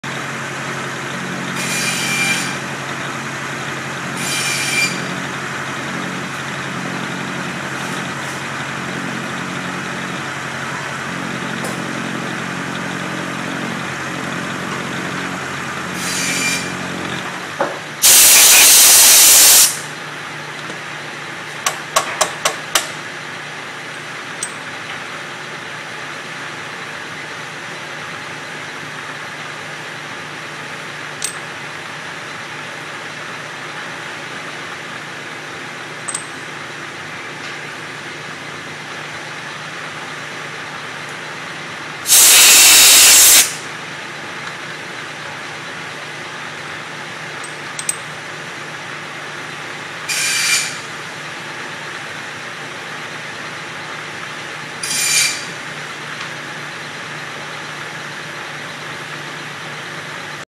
Miz Raven working the Bridgeport milling machine
Hold on to your crash helmets this video may be too exciting! Miz raven working the mill. Well my hair looks kool!